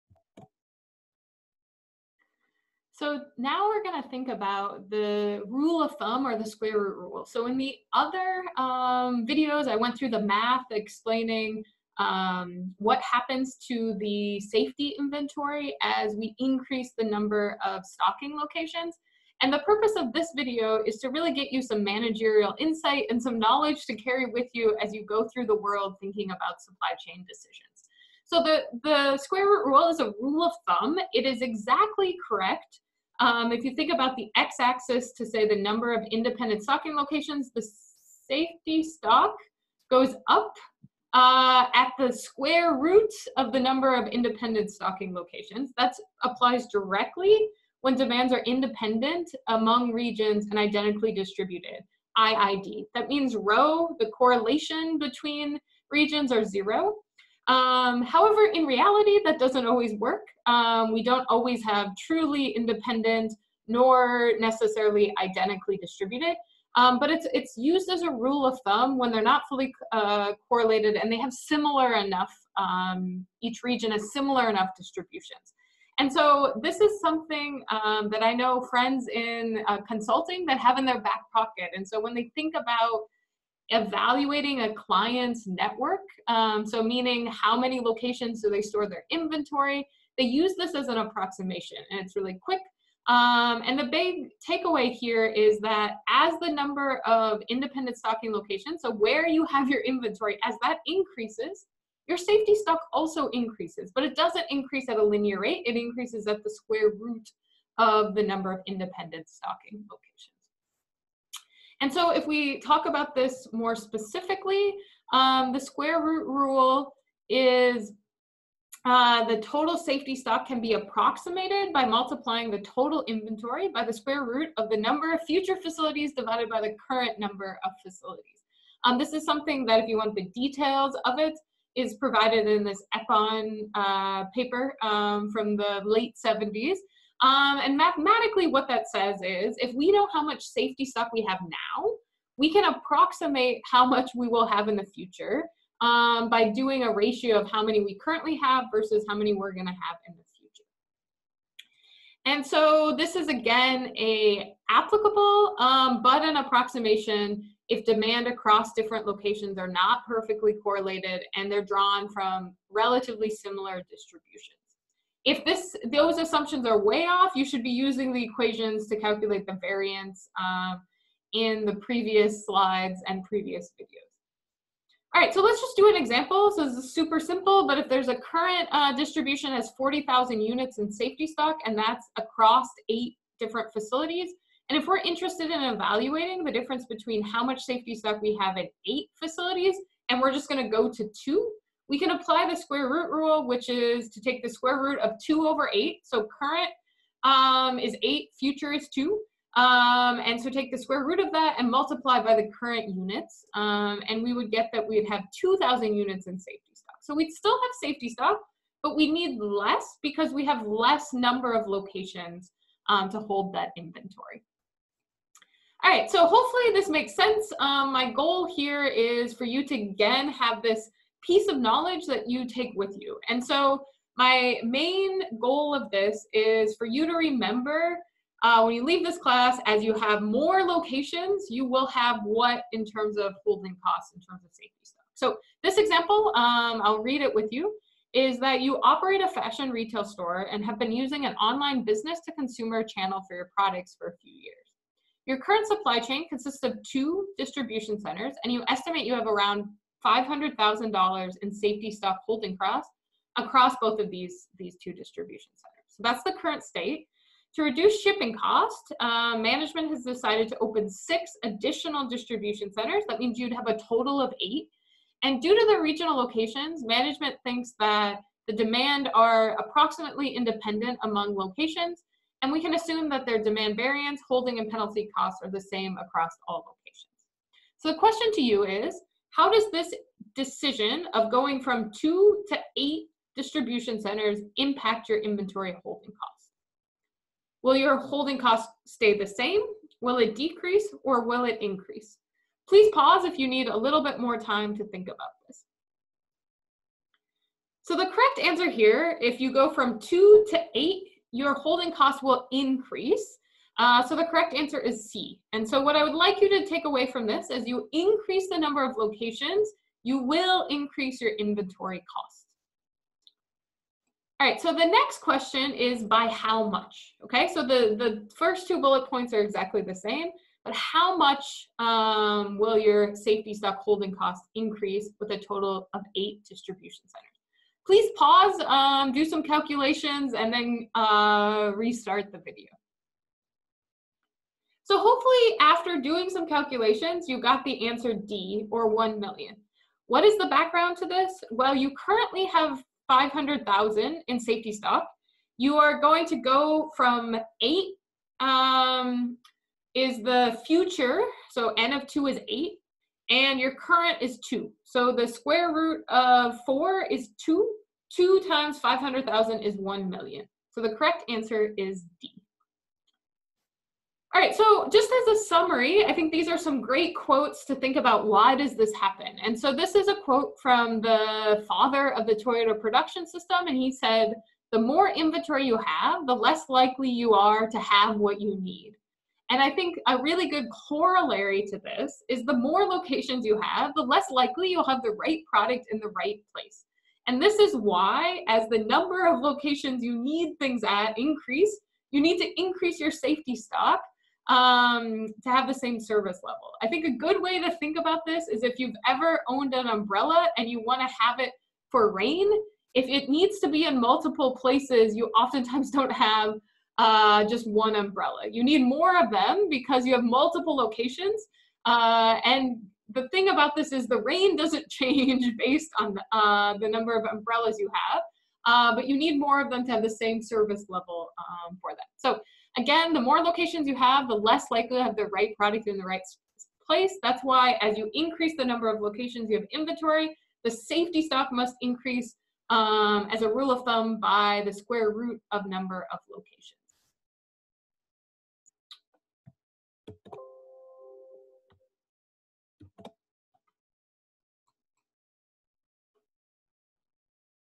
Square Root Rule for Multiple Stocking Locations
This video presented by Jen Pazour is part of the course ISYE 4210 Design and Analysis of Supply Chains taught at Rensselaer Polytechnic Institute. More details about Jen's teaching and research interests can be found